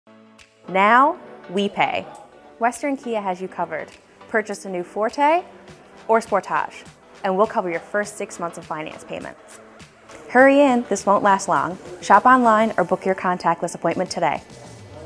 Now WE Pay - at Western Kia
Now WE Pay. We'll pay your first 6 months finance payments when you buy a new Forte or Sportage this month. That's up to a $4,300 value. Hurry in! This won't last long. KIA has you covered - see dealer for details.